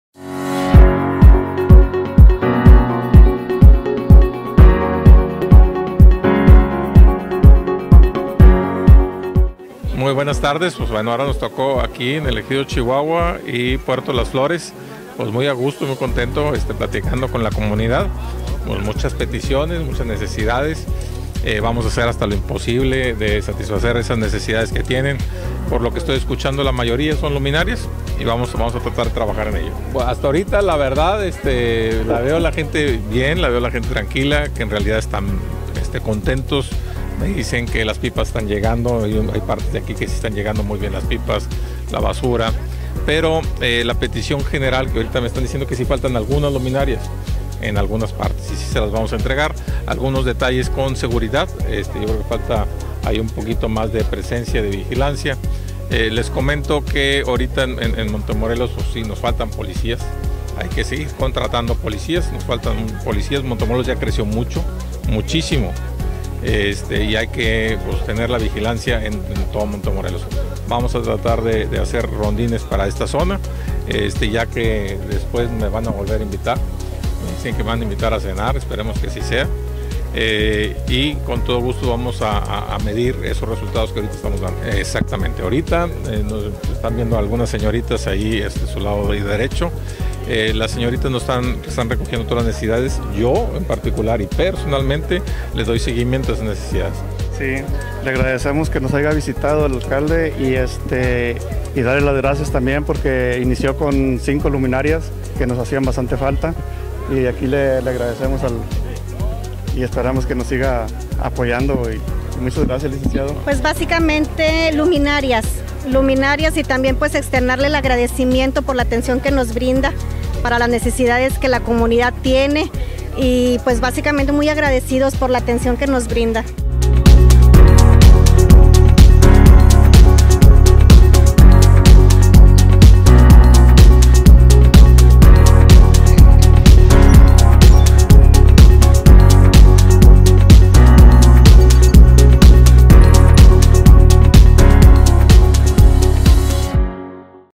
ESCUCHA LAS PETICIONES DE LOS VECINOS DEL EJIDO CHIHUAHUA Y DE LA COMUNIDAD PUERTO LAS FLORES
ALCALDE DE MONTEMORELOS N.L. ESCUCHA LAS PETICIONES DE LOS VECINOS  DEL EJIDO CHIHUAHUA Y DE LA COMUNIDAD PUERTO LAS FLORES